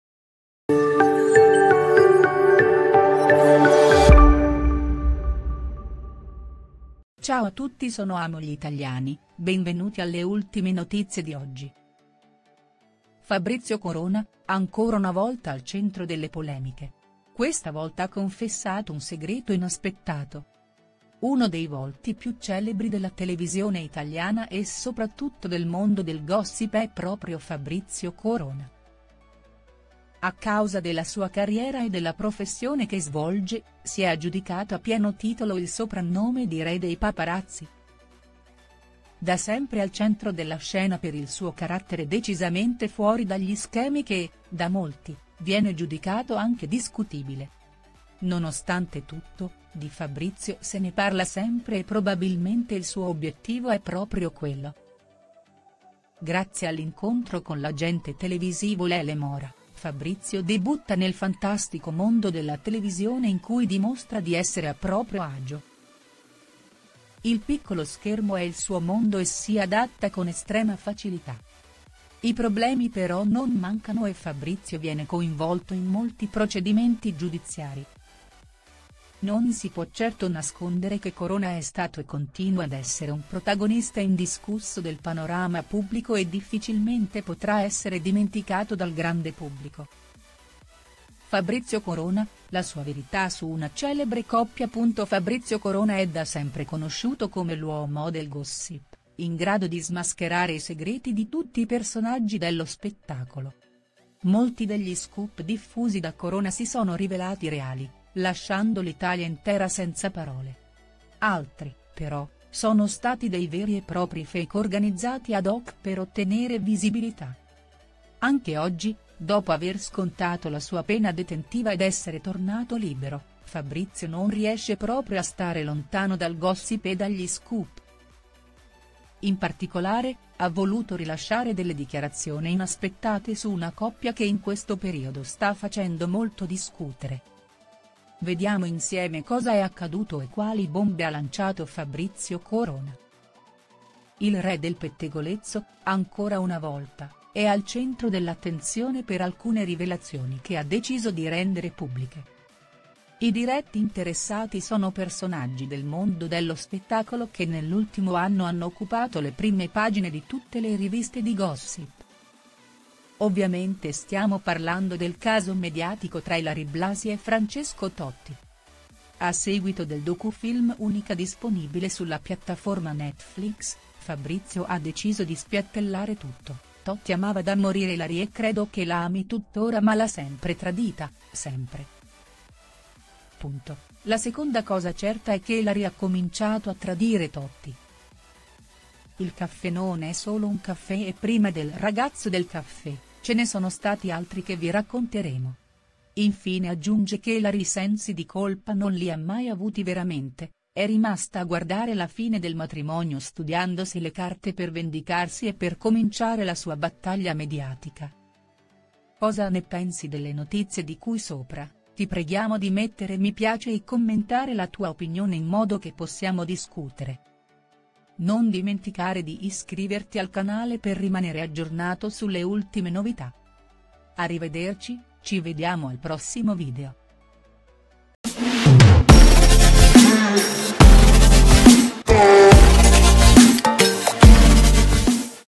Fabrizio Corona ancora una volta spiattella tutto il Re dei paparazzi ‘sputa’ un segreto vergognoso
Uno dei volti più celebri della televisione italiana e soprattutto del mondo del gossip è proprio Fabrizio Corona. A causa della sua carriera e della professione che svolge, si è aggiudicato a pieno titolo il soprannome di “Re dei paparazzi“.

Da sempre al centro della scena per il suo carattere decisamente fuori dagli schemi che, da molti, viene giudicato anche discutibile. Nonostante tutto, di Fabrizio se ne parla sempre e probabilmente il suo obiettivo è proprio quello.

Grazie all’incontro con l’agente televisivo Lele Mora, Fabrizio debutta nel fantastico mondo della televisione in cui dimostra di essere a proprio agio. Il piccolo schermo è il suo mondo e si adatta con estrema facilità. I problemi però non mancano e Fabrizio viene coinvolto in molti procedimenti giudiziari.

Non si può certo nascondere che Corona è stato e continua ad essere un protagonista indiscusso del panorama pubblico e difficilmente potrà essere dimenticato dal grande pubblico.